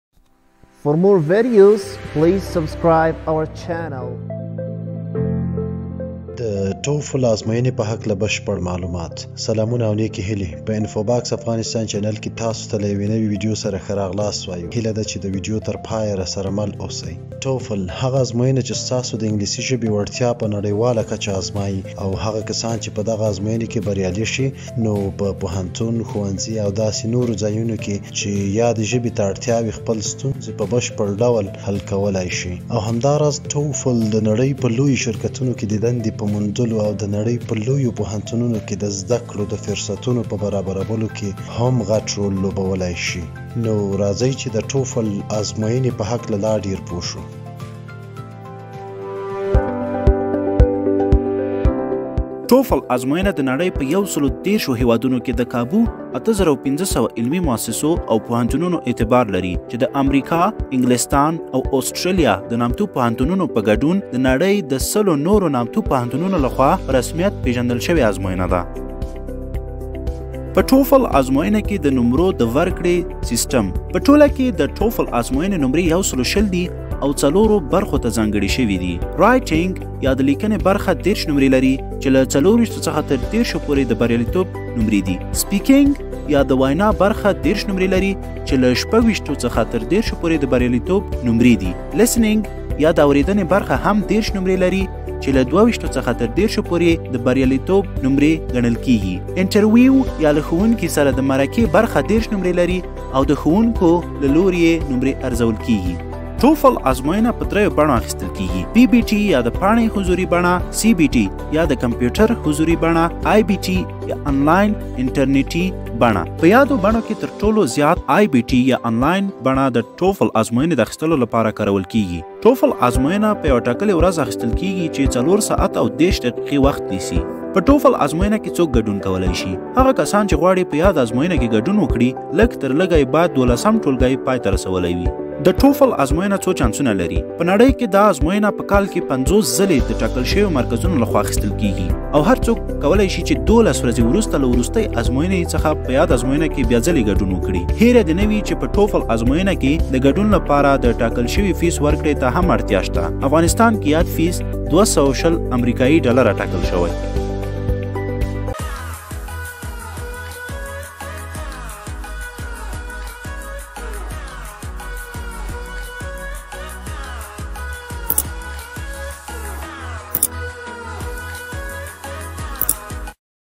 TOEFL_Introduction/د ټوفل آزموینې په هکله بشپړ معلومات
په دغه ویډیو کې به د ټوفل نړیوالې آزموینې په هکله په بشپړ ډول معلومات درکړو